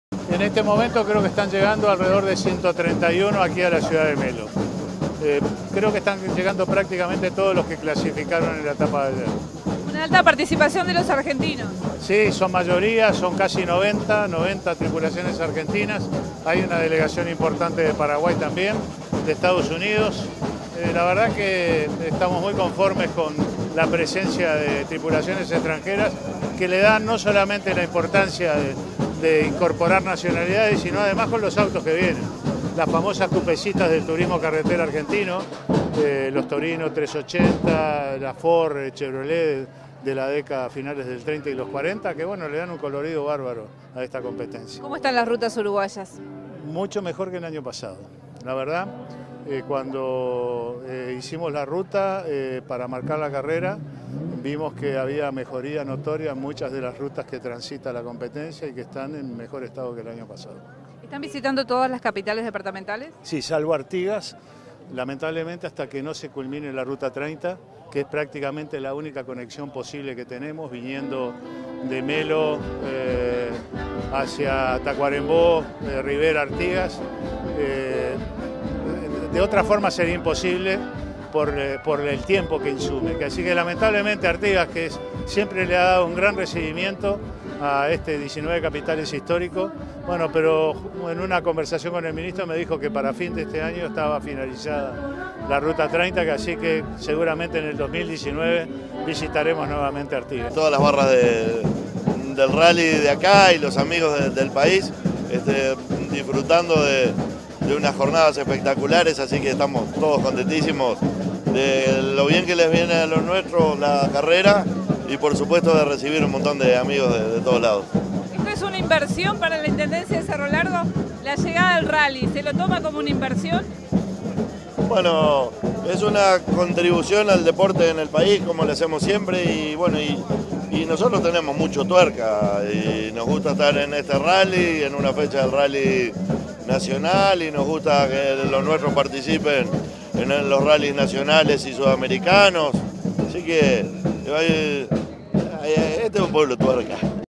Automovilismo 131 autos clásicos visitaron
En el marco del Rally 19 Capitales Histórico en su 15 Edición la capital de Cerro Largo los recibió con una cálida bienvenida!